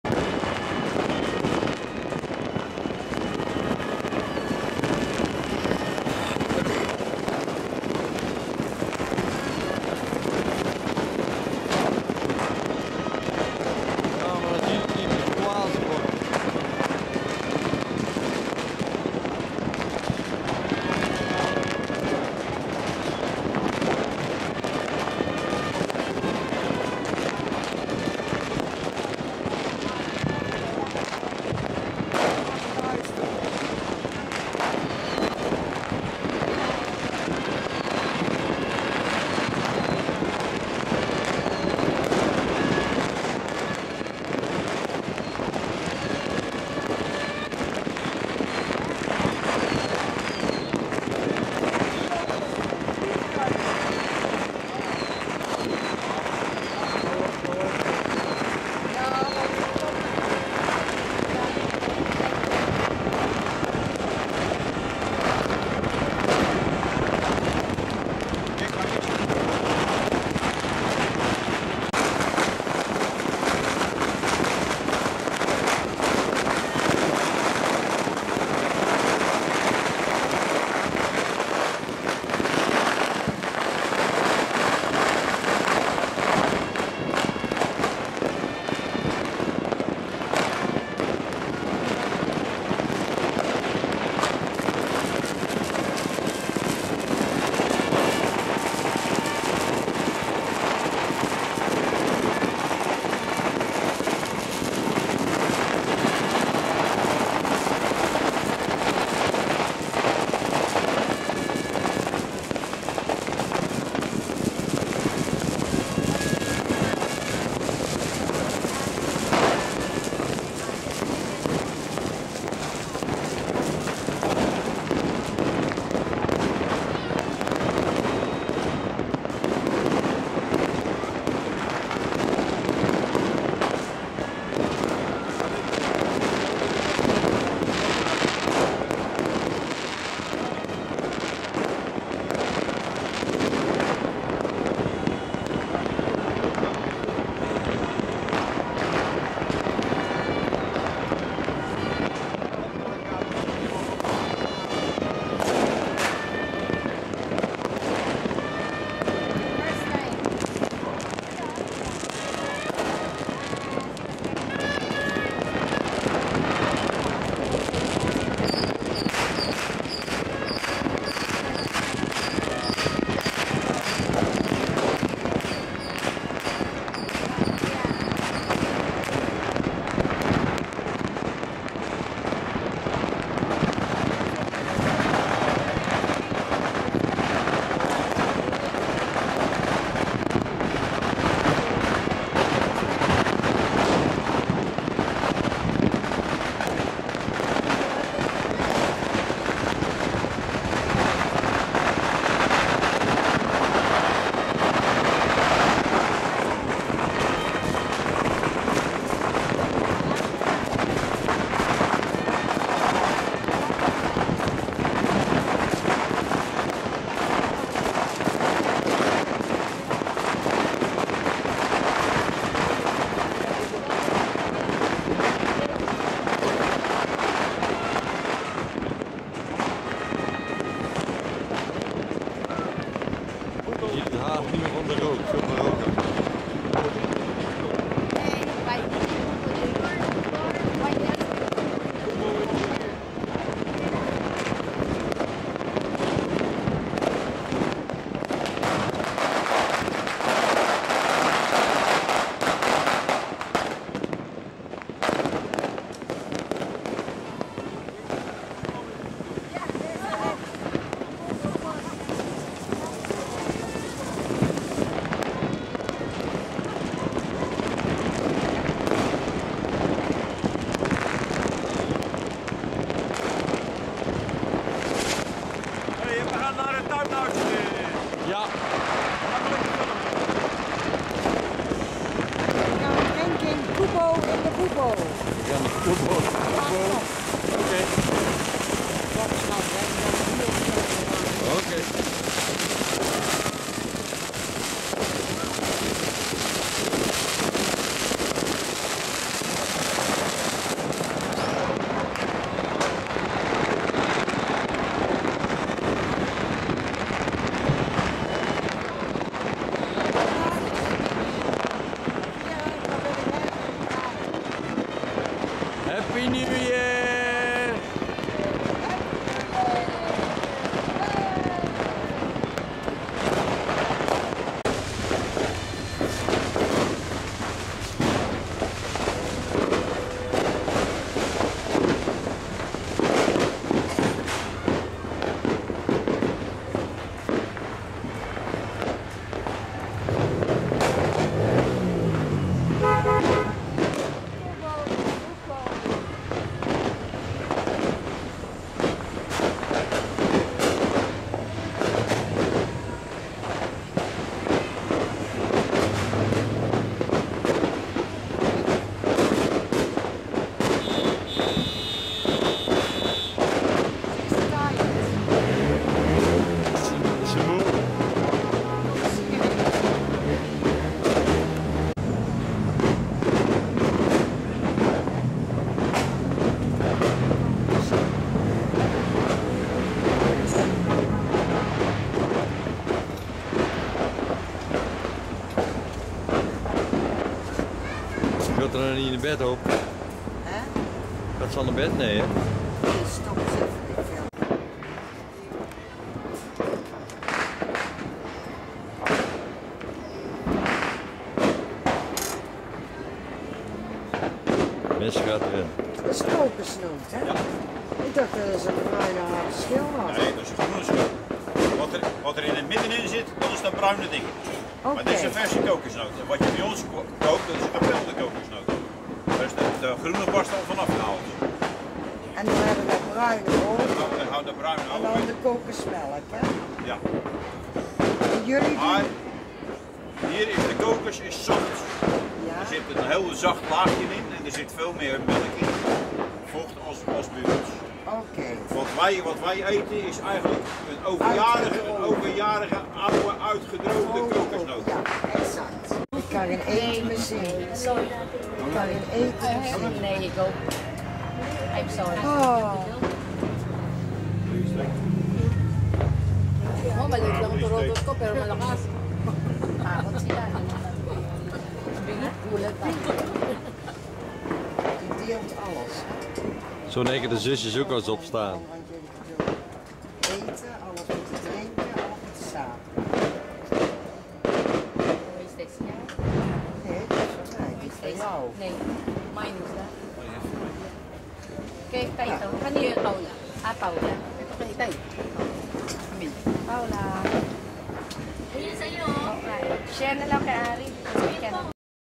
Phil Fonds Oud & Nieuw-viering 2009-2010 in LEBD, Metro Manilla
Nieuwjaars-Vuurwerk over Metro Manilla gezien vanuit het Phil Fonds LEBD centrum in Pasig City 2009/2010